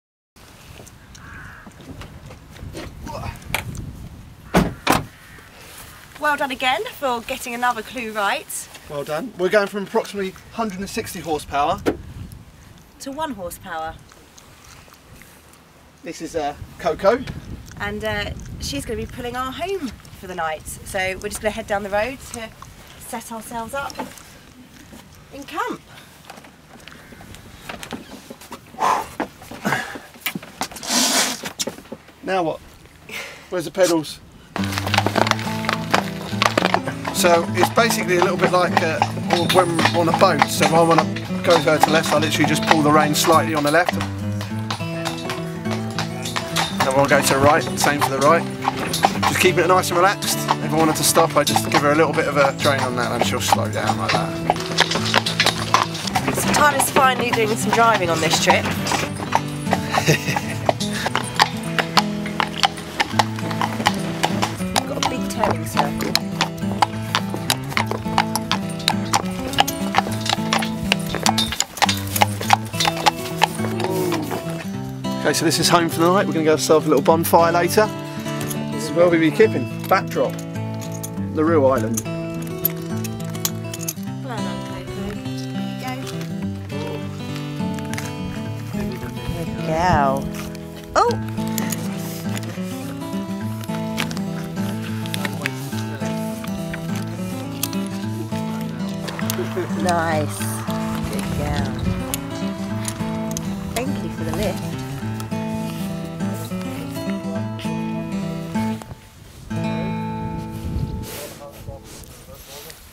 Man, woman, horse, happy
It's Tyler and Amy's Ireland Mystery tour. It's got wheels, it's got four legs, it's got a penchant for boiled sweets. This is life under a blanket of stars in our very own horse caravan.